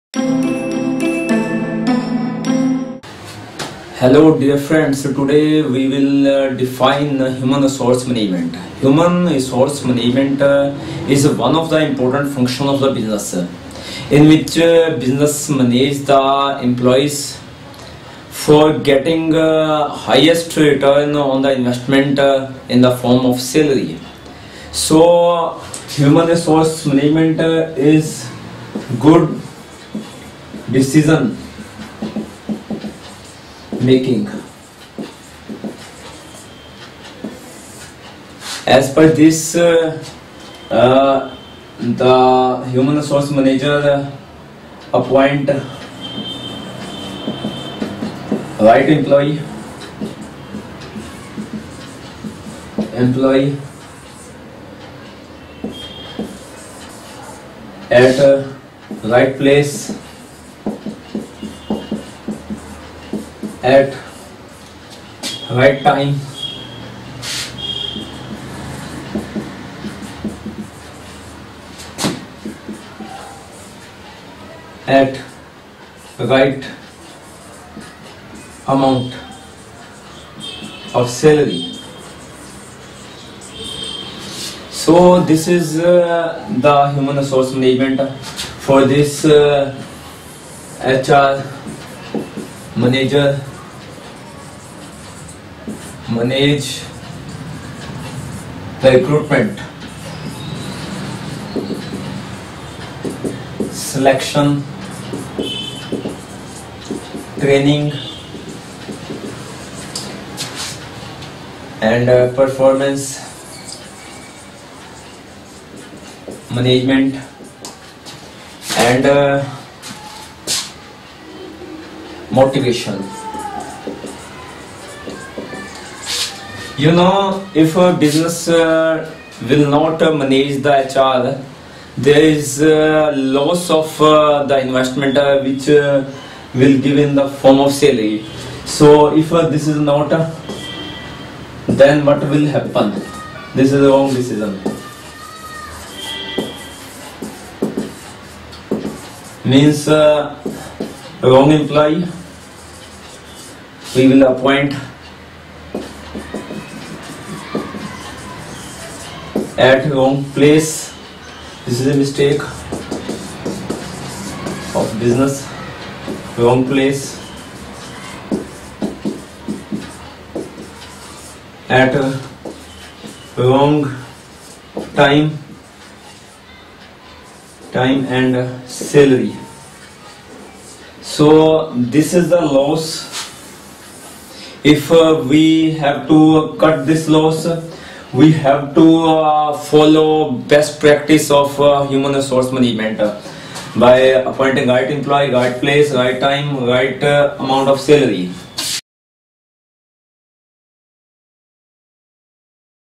Introduction of Human Resource Management : Svtuition
From this first lecture of Human resource management subject, you will learn the meaning of HRM.